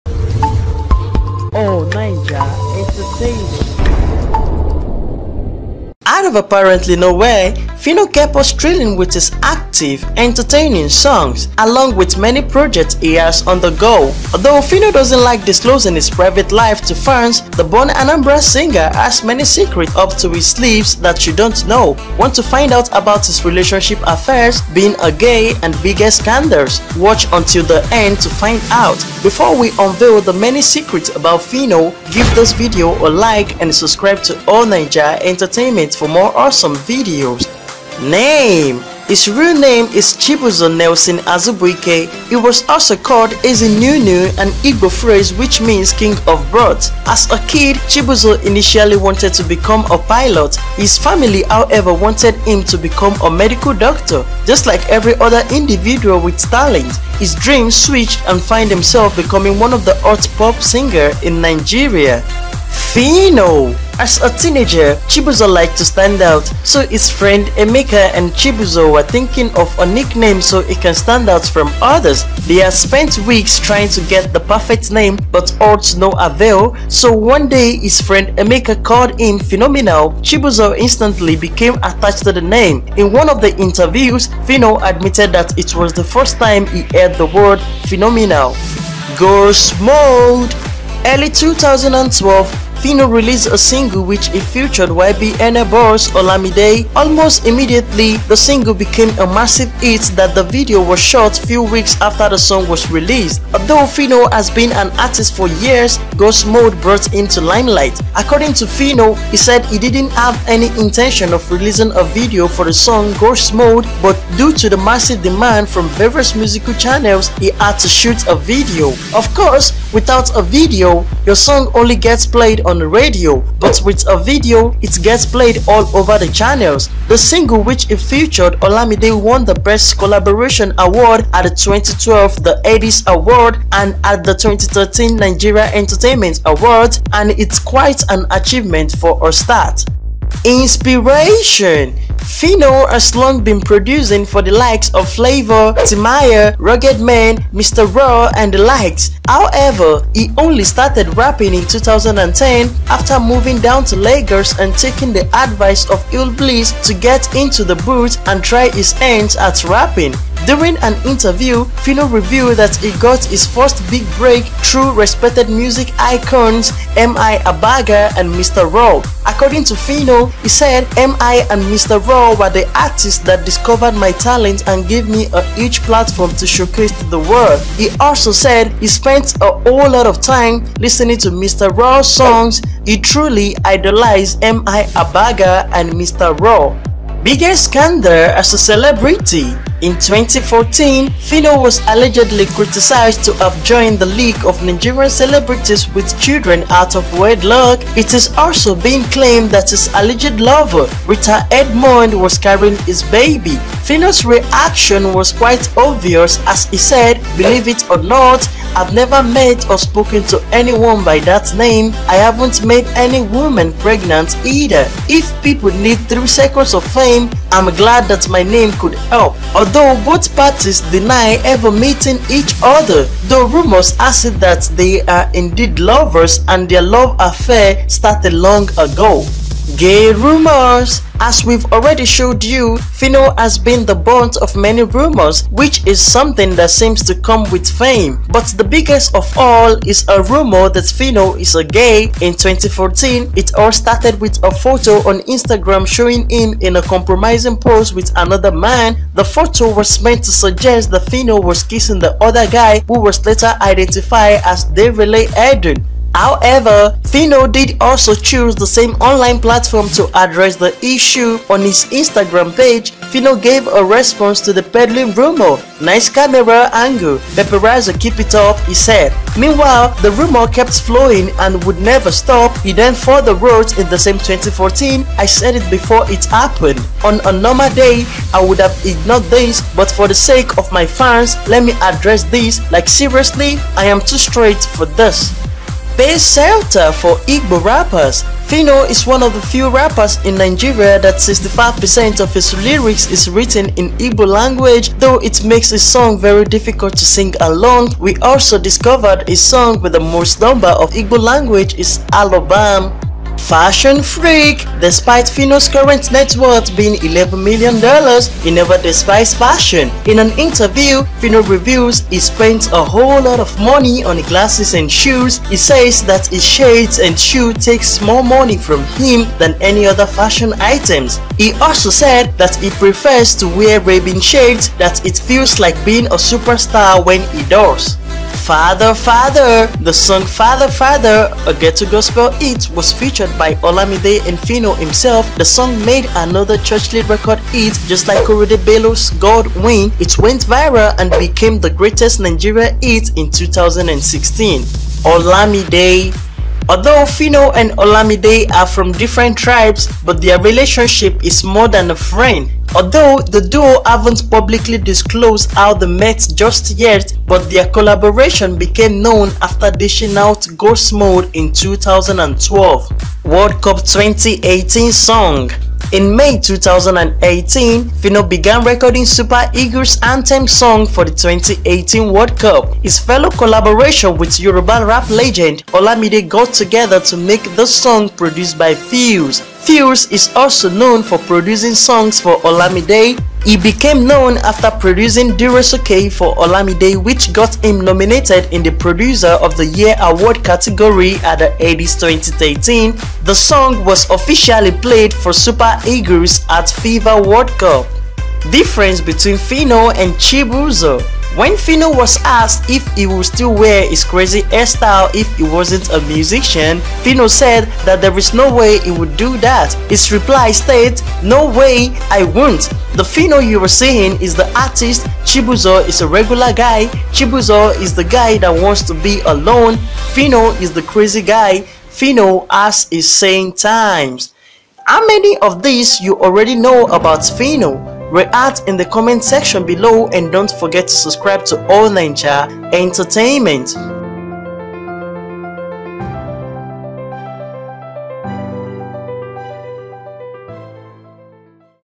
15 Things You Didn't Know About Phyno
Although Phyno Doesn't Like Disclosing His Private Life To Fans, The Born Anambra Born Singer Has Many Secrets Up To His Sleeves That You Don't Know.

Want To Find Out About His Relationship Affairs? Being A Gay And His Biggest Scandals? Watch Until The End To Find Out.